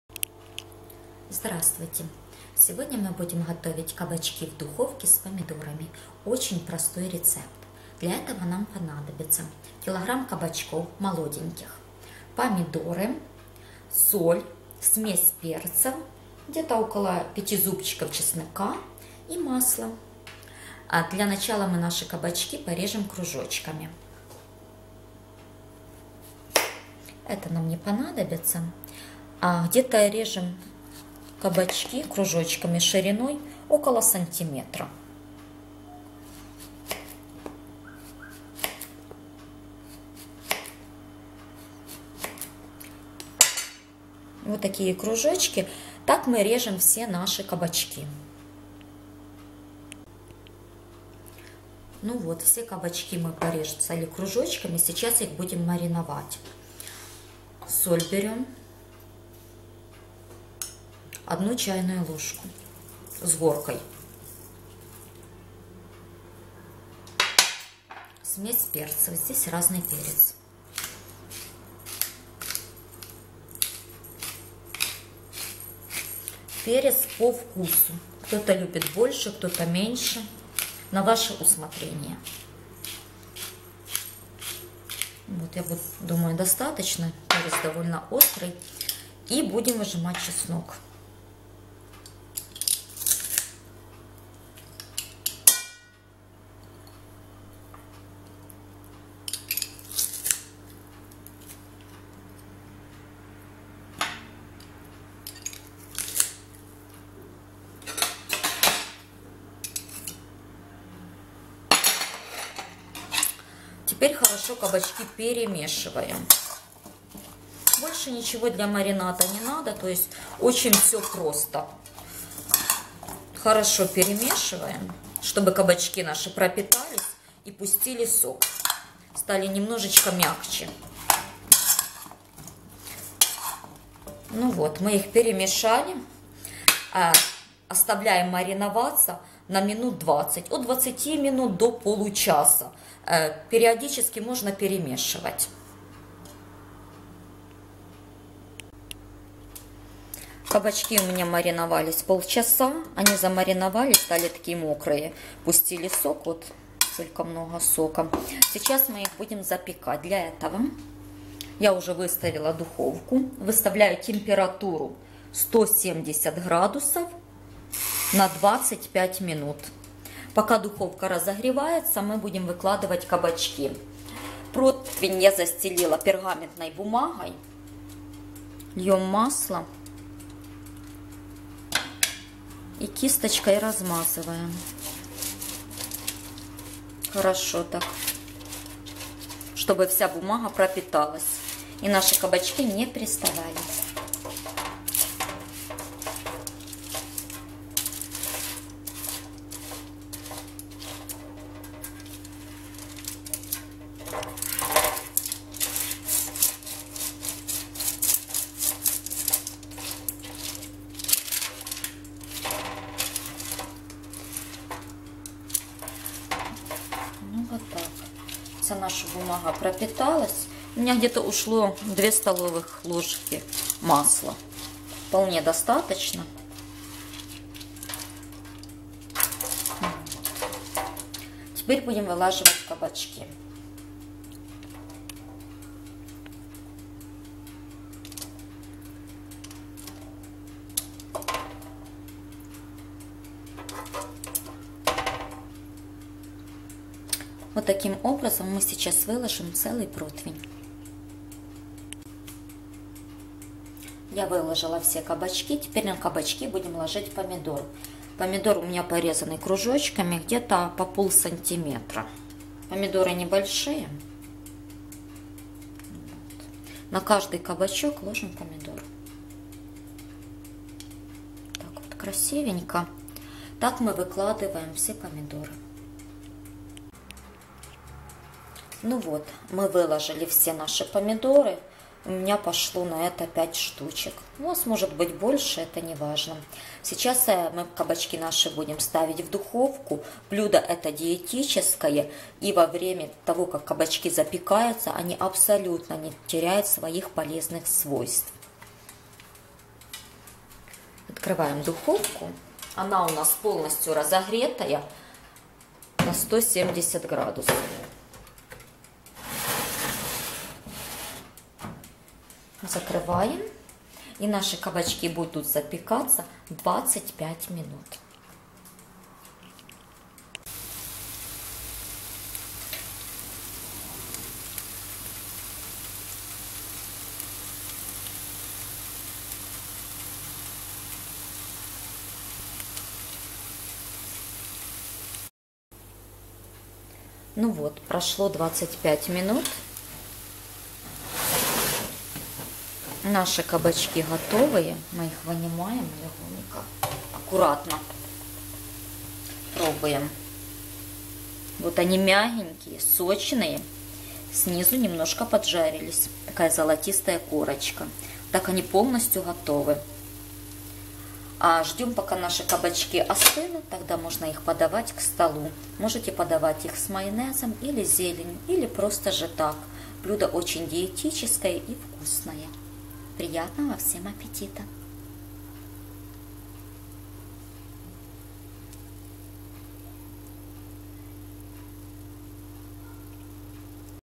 Диетические Кабачки в Духовке с Помидором! Полезно и Вкусно!
Диетические кабачки в духовке с помидорой,очень полезно